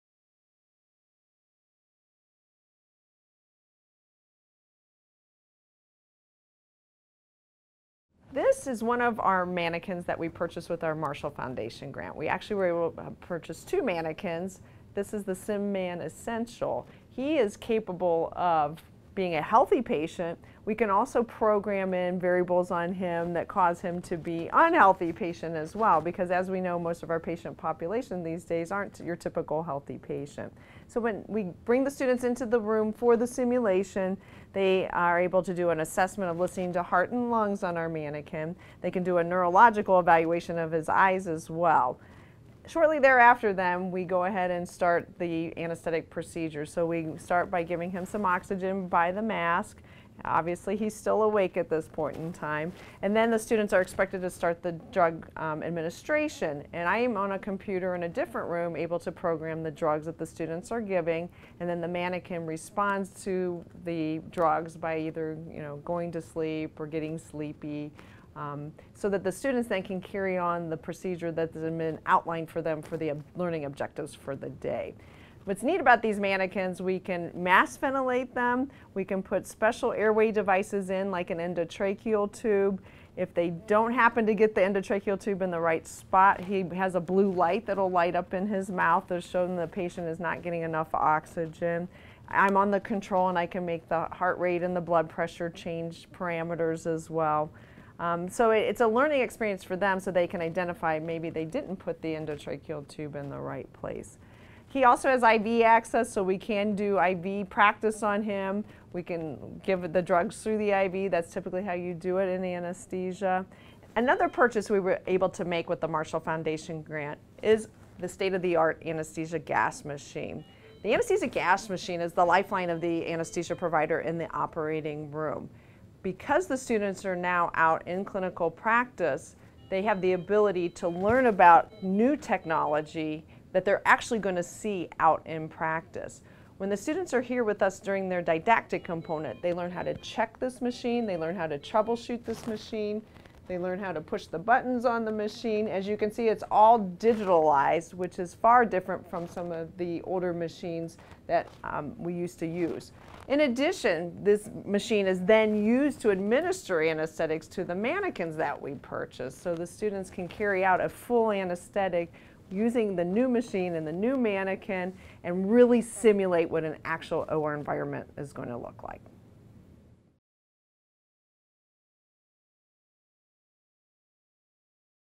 Inside the OR: Simulation for Nurse Anesthetist Students
The cutting-edge simulation technology was made possible through a grant by the Marshall Foundation.